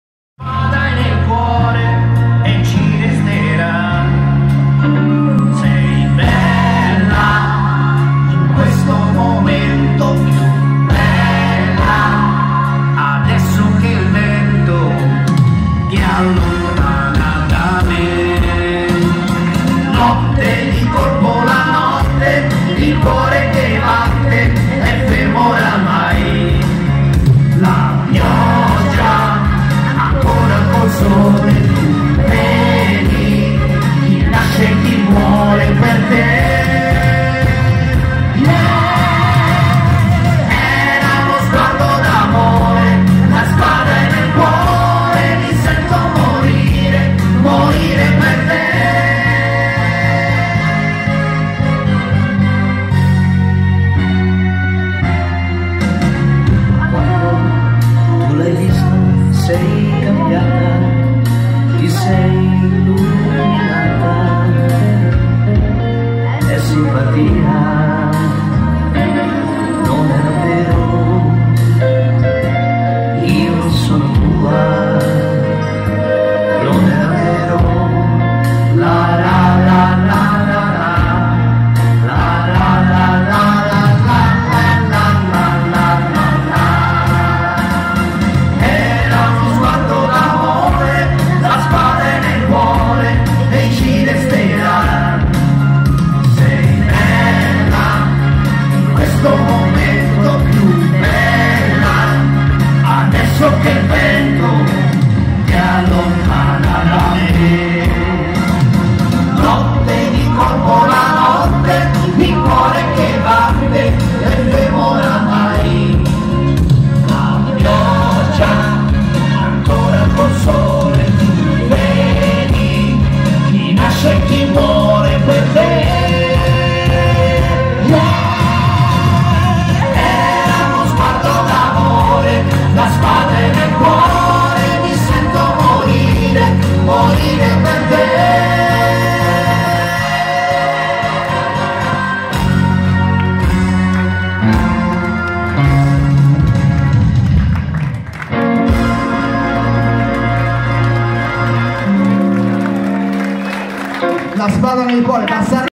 SERATA CONCERTO "LITTLE TONY FAMILY"
SERATA CONCERTO DELLA "LITTLE TONY FAMILY"
AL RISTO DANCE "LA BARACCA VILLAGE"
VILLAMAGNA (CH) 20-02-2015